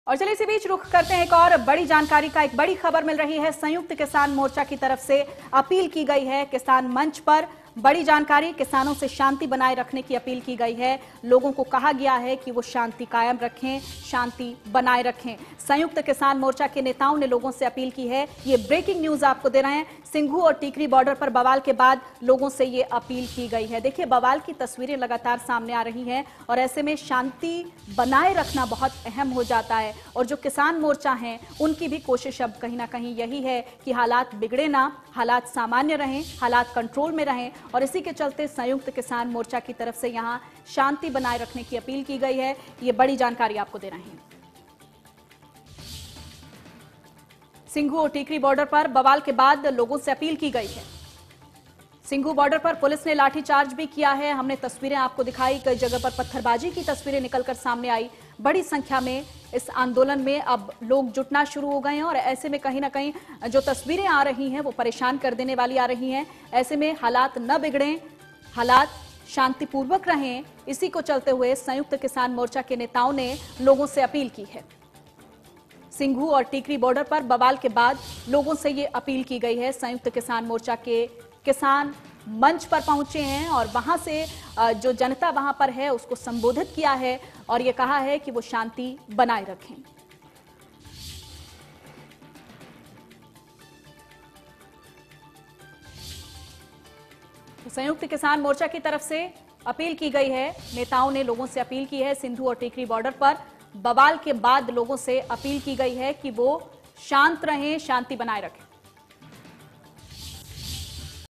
संयुक्त किसान मोर्चा के नेताओं ने लोगो से शांति बनाये रखने की अपील I India News Haryana
संयुक्त किसान मोर्चा के नेताओं ने लोगो से शांति बनाये रखने की अपील I India News Haryana I India News Haryana